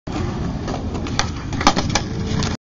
kickflip step up
kickflip up the step up at wirth park